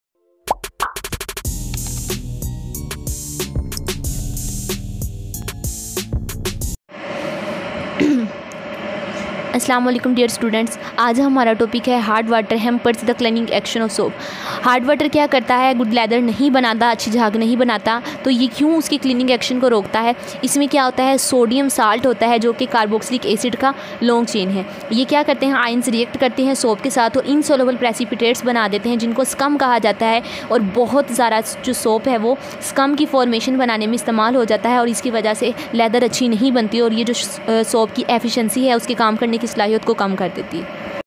Class 10th chemistry| Define and Discus Hard water Hampers The Cleaning action of Soap|Math Scholar
Class 10th Chemistry| Define and Discus Hard water Hampers The Cleaning action of Soap in Urdu and Hindi|Math Scholar|